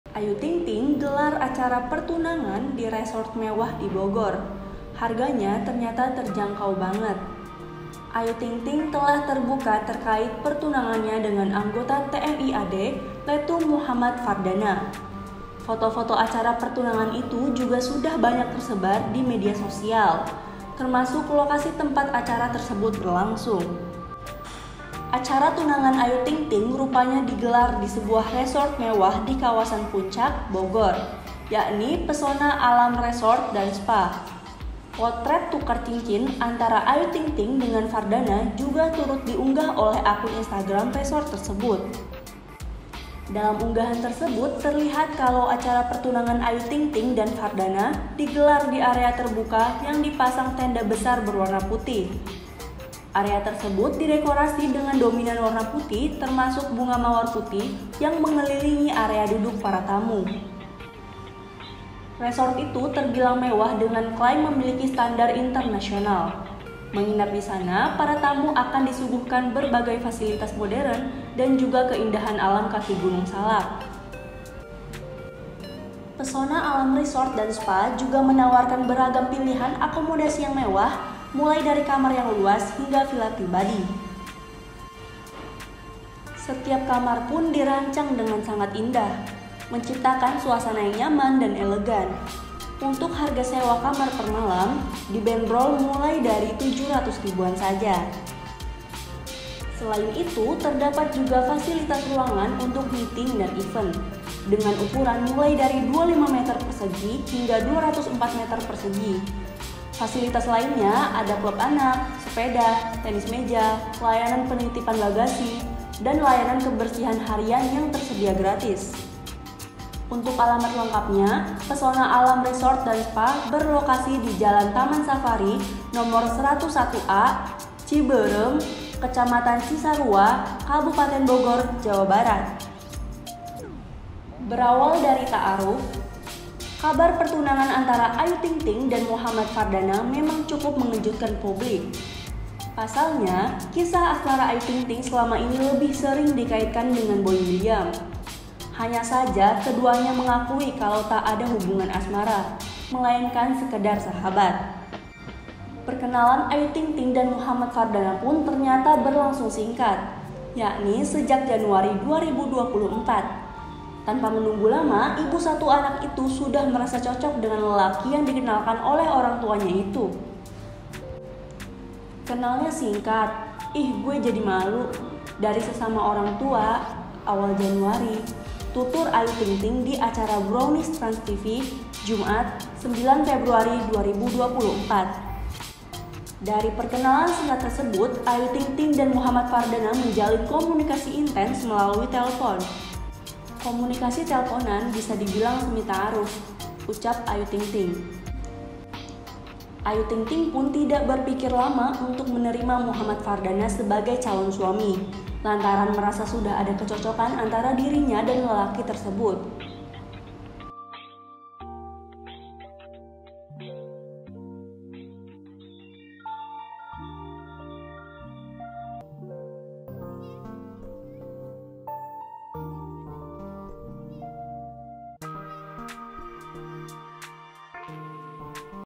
Ayu Ting Ting Gelar Acara Pertunangan di Resort Mewah di Bogor, Harganya Ternyata Terjangkau Banget
Ayu Ting Ting telah terbuka terkait pertunangannya dengan anggota TNI AD Lettu Muhammad Fardana.

Acara tunangan Ayu Ting Ting rupanya digelar di sebuah resor mewah di kawasan Puncak, Bogor, yakni Pesona Alam Resort & Spa. Potret tukar cincin antara Ayu Ting Ting dengan Fardana juga turut diunggah oleh akun Instagram resort tersebut.

Dalam unggahan tersebut, terlihat kalau acara pertunangan Ayu Ting Ting dan Fardana digelar di area terbuka yang dipasang tenda besar berwarna putih. Area tersebut didekorasi dengan dominan warna putih, termasuk bunga mawar putih yang mengelilingi area duduk para tamu.

VO/Video Editor: Sosmed/Vila
ニニニニニニニ